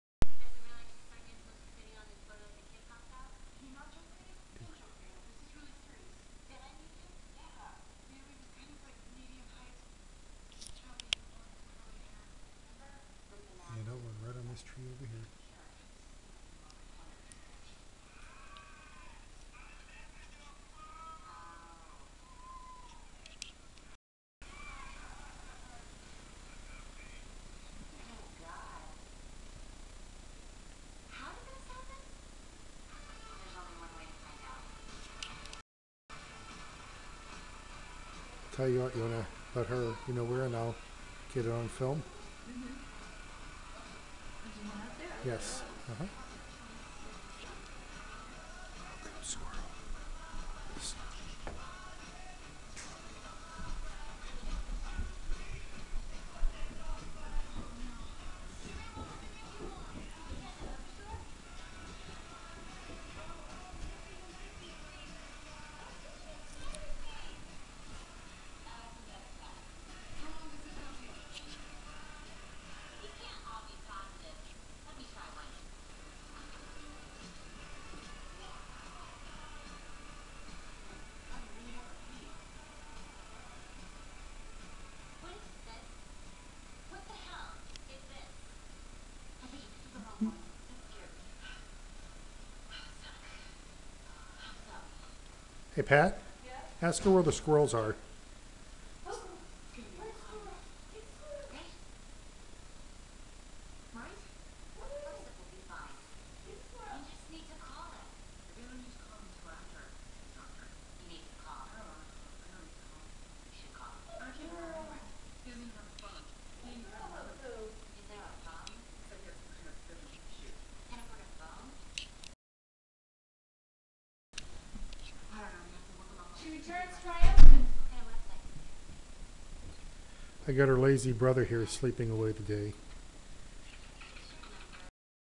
Brave Poodle! Bad squirrels
Poodle defends home against squirrels